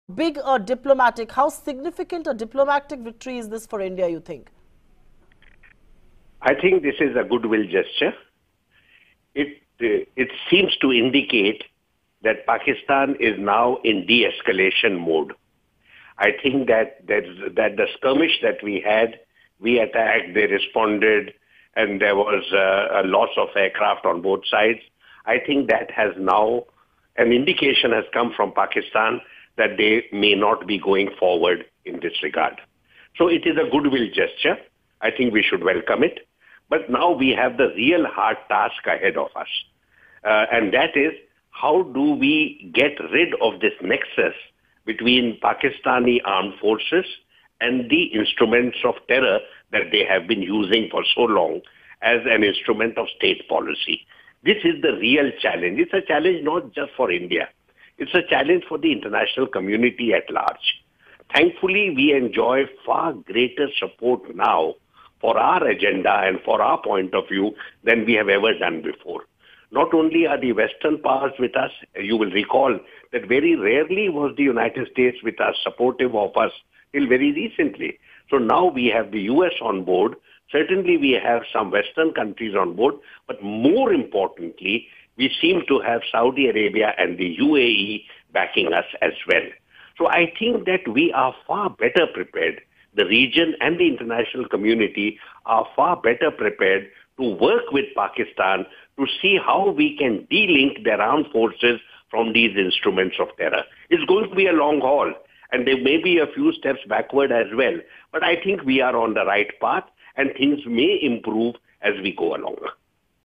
Task ahead for India is to get rid of Pak army, terror nexus: Talmiz Ahmad
Releasing IAF Pilot is a goodwill gesture by Pakistan and we must welcome it. Now the task that lies ahead is that how do we get rid of the nexus between Pak army and the terror outfits: Talmiz Ahmad.